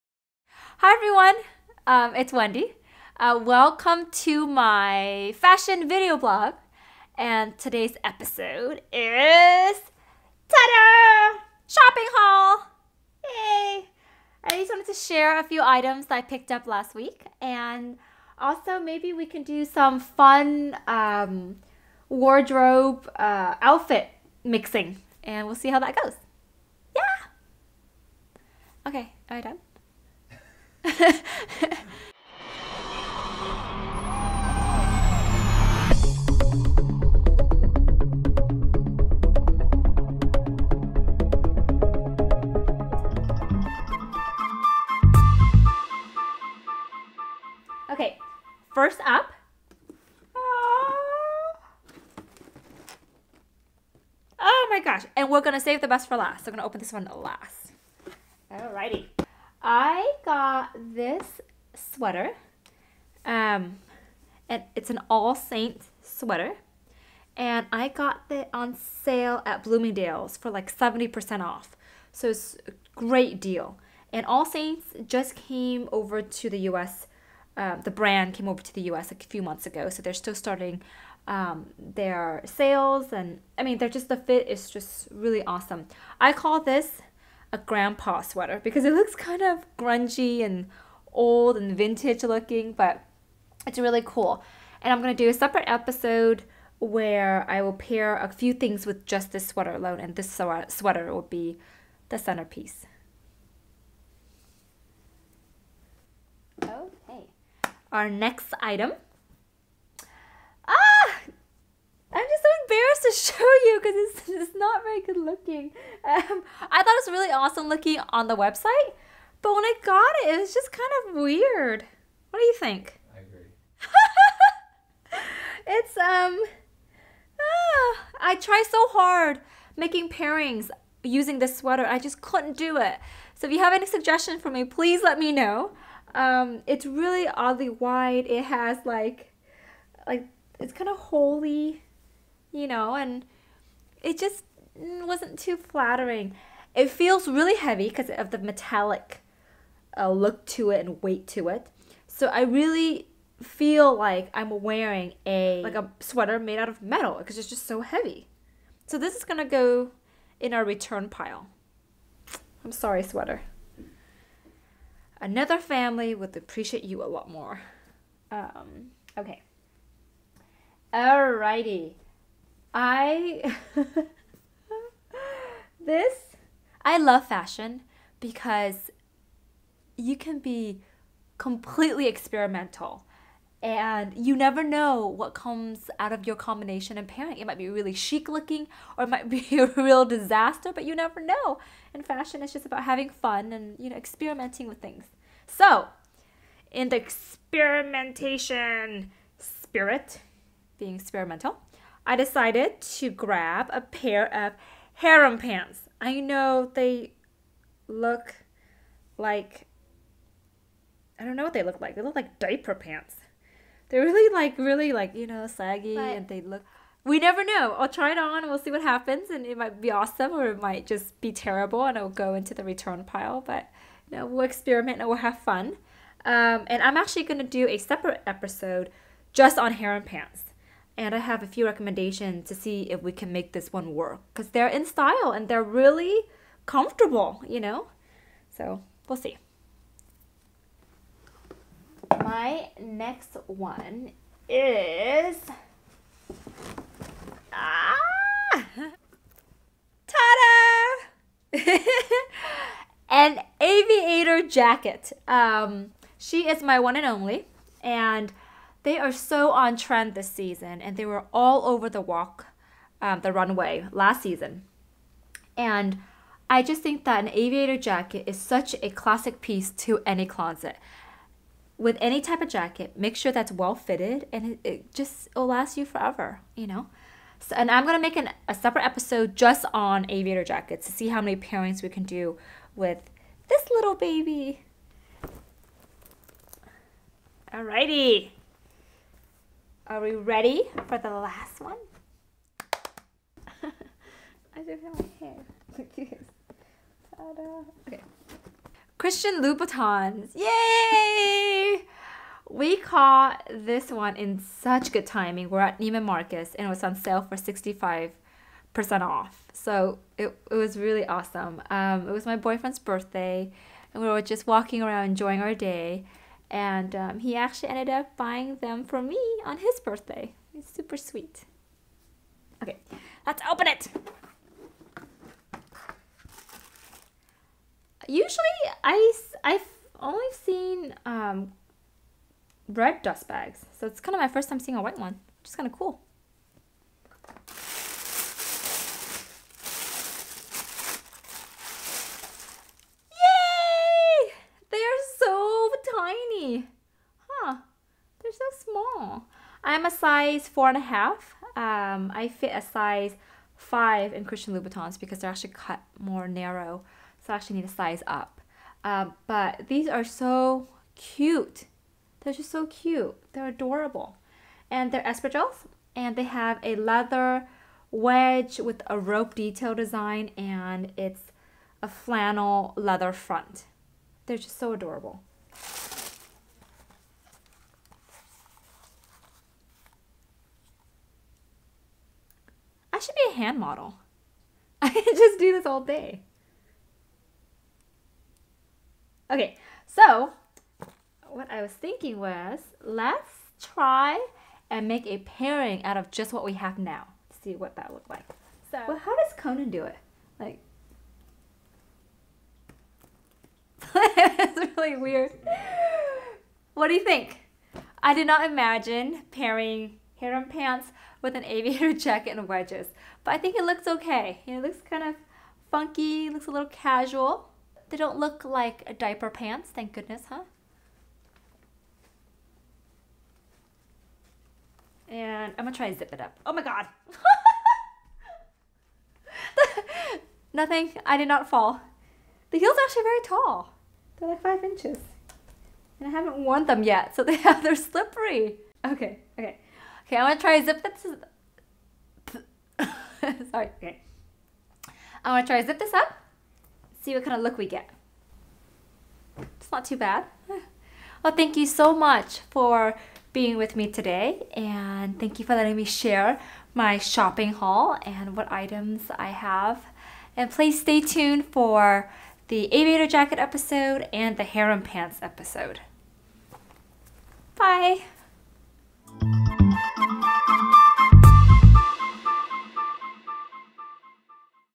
Shopping Haul: Christian Louboutin & more
Hi Everyone!  I just want to share with you a few items I picked up this week.  I've been wanting to experiment with harem pants and aviator jackets for awhile, and finally I found a pair that are super comfortable and a jacket that fits seamlessly.  And saving the best for last, I got a pair of Christian Louboutins!   From this shopping haul, I'm going to do three separate episodes highlighting: harem pants, aviator jackets, and grandpa sweaters.